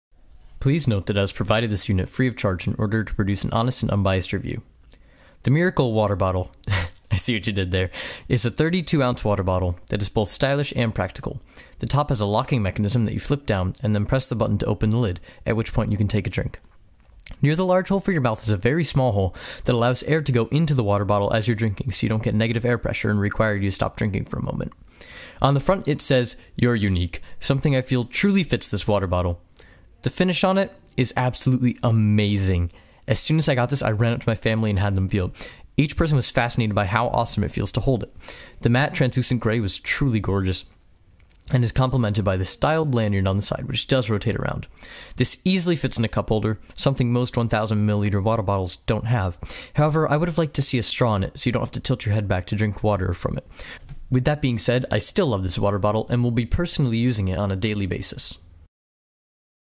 MIRACOL Water Bottle YouTube
This is my review of the "Miracol Sports Water Bottle, 32oz Large - Eco-Friendly Tritan Co-Polyester Plastic - with Flip Top Lid & Adjustable Strap - BPA Free - Best for Gym, Cycling, Camping, Running"